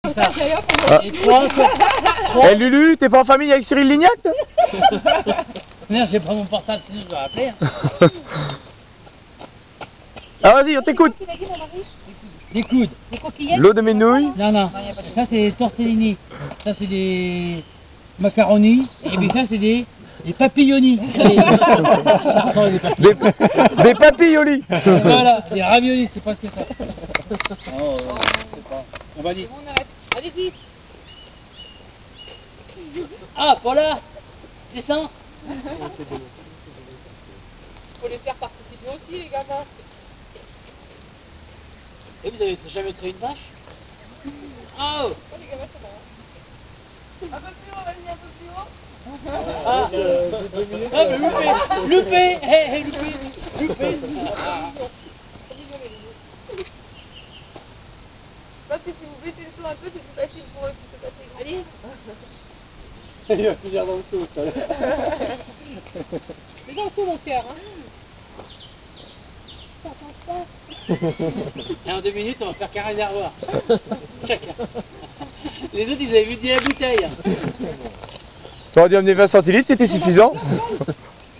Gué de Douy
Lulu invente de nouvelles pates et se fait mouiller